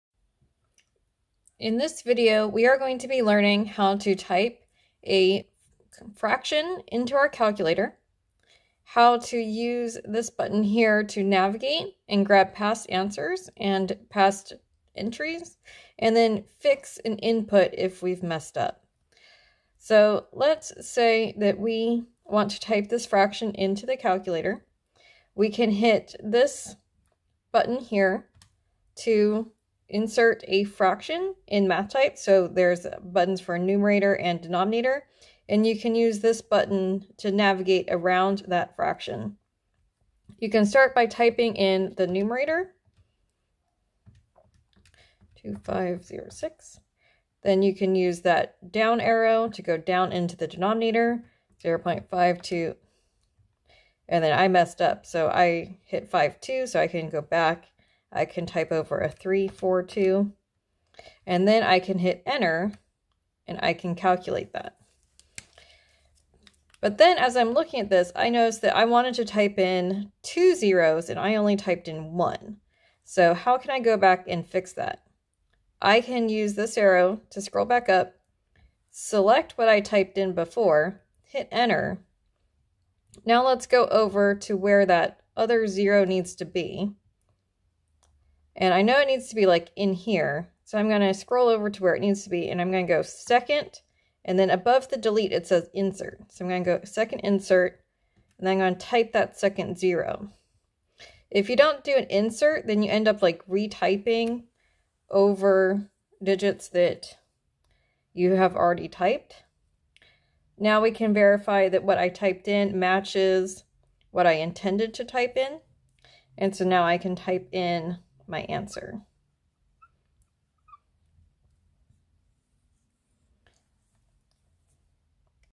TI-36X Pro - Navigation, Fractions, Insert
How to use the TI-36X Pro Calculator:

Aimed for Students in Science College Courses.

Navigation
Selecting and fixing past entries
Inserting a digit